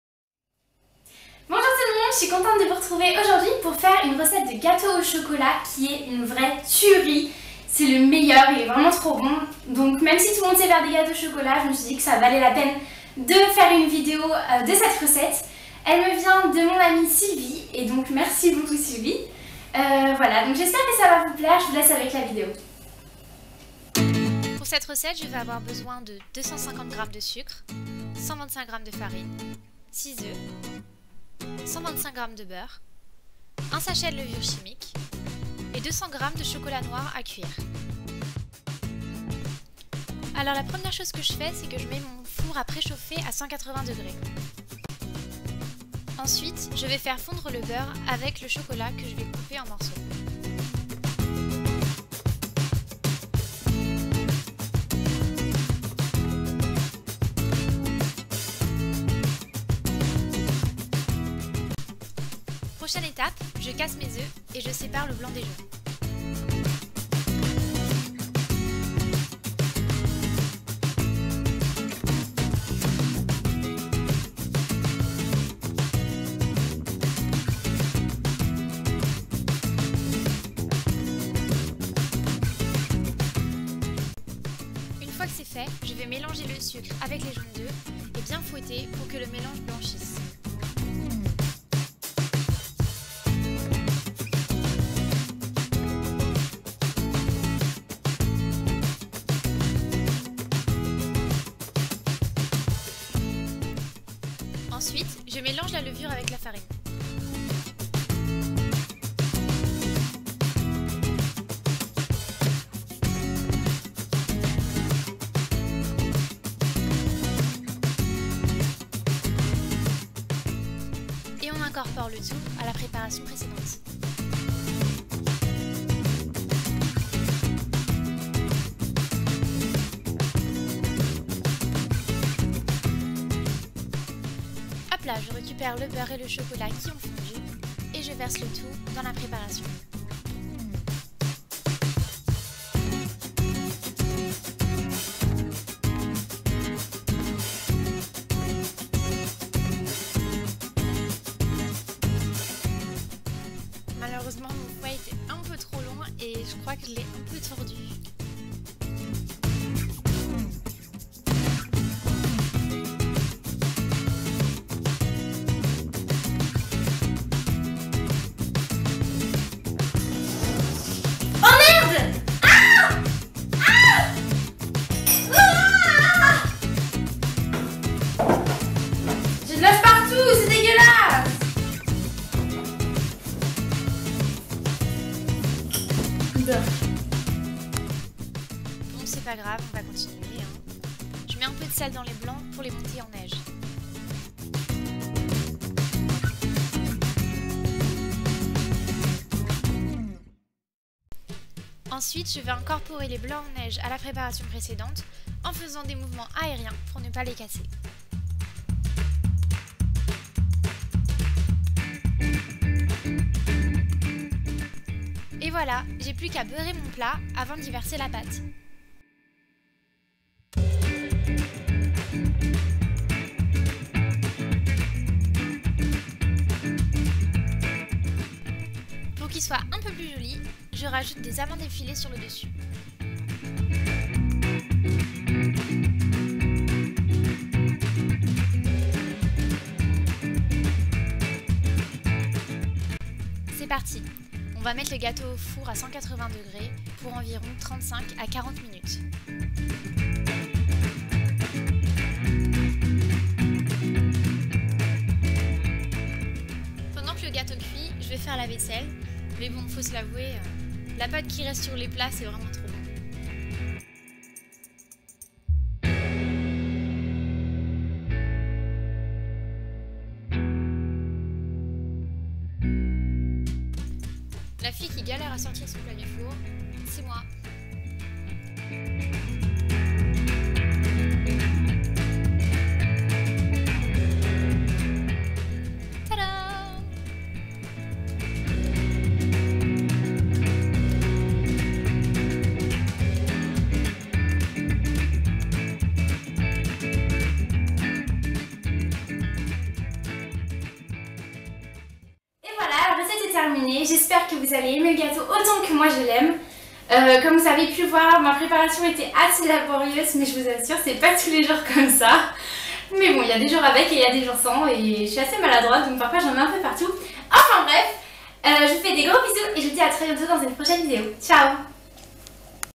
[Recette °3] THE gâteau au chocolat ♥
Il est tellement bon que j'étais obligée d'en faire une vidéo! Il ne fait que des heureux, essayez-le :)
Bisous ♥

Retrouvez-moi sur